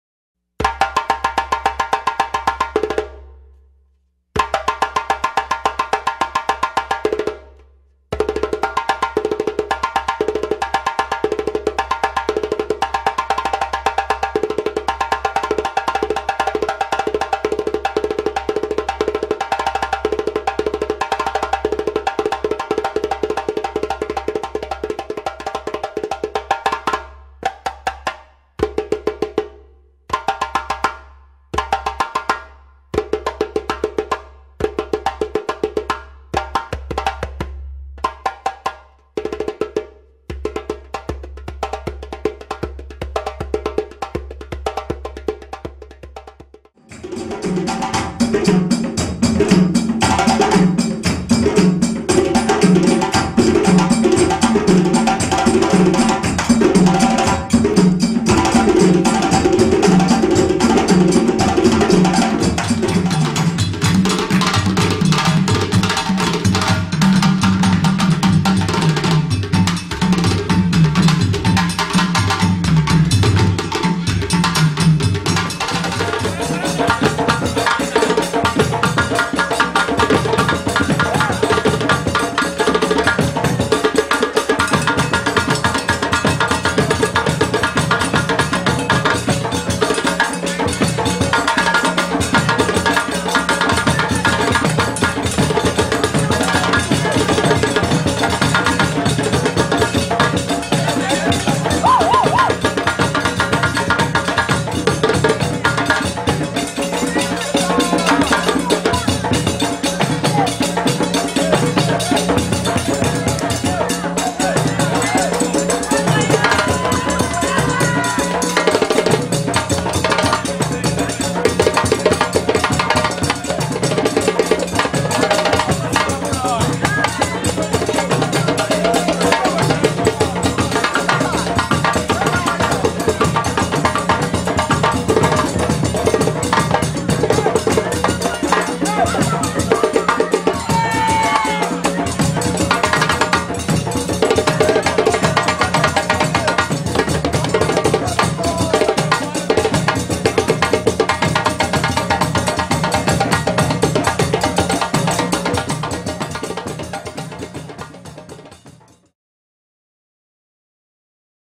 m'bemba bangoura 01.mp4
workshop youssouf koumbassa and m'bemba bangoura in israel
9-12 april 2012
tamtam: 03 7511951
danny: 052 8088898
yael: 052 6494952